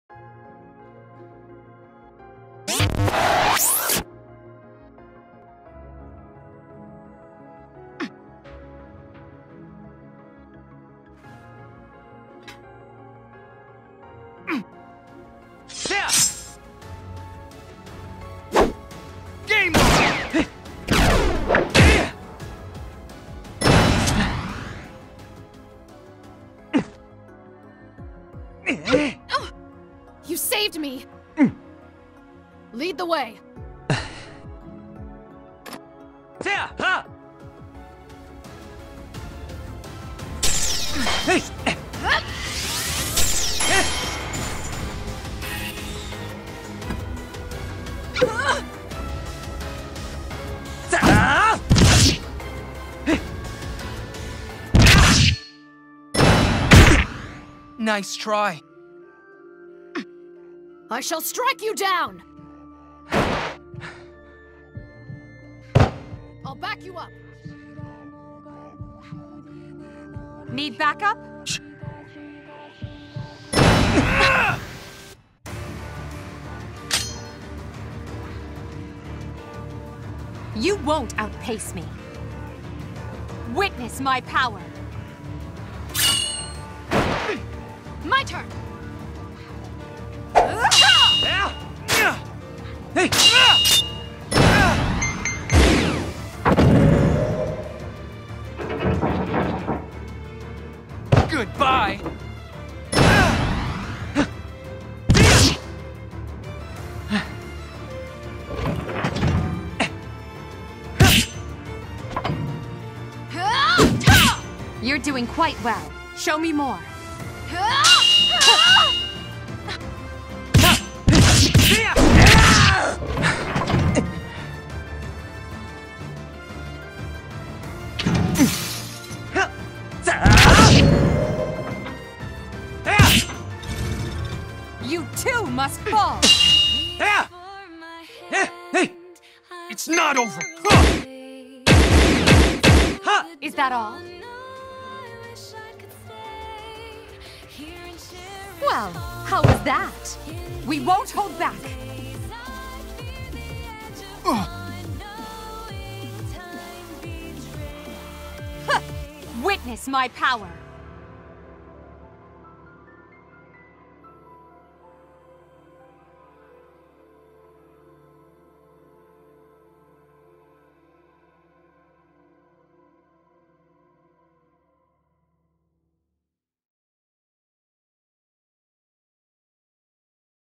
Dark Pit's Lair
Link infiltrates an evil lair to save Lucina